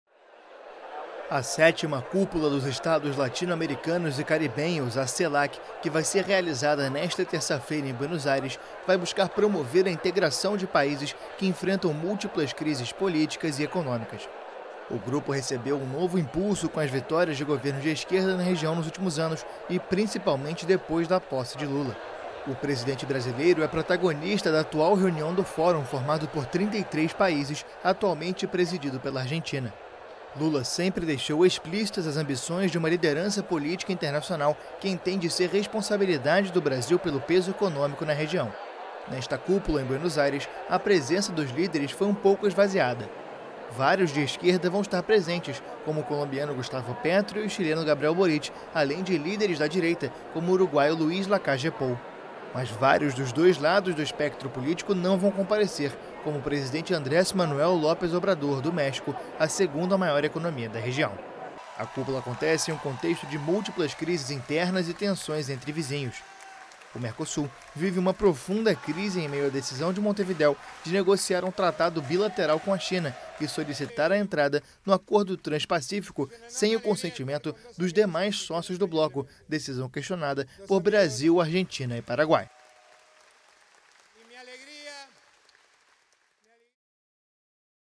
Com protagonismo do Brasil, Celac ganha força | AFP
Com o protagonismo do Brasil, a Comunidade dos Estados Latino-Americanos e Caribenhos ganhou força e vai ter nesta terça-feira, em Buenos Aires, uma edição para fortalecer a região no cenário econômico global.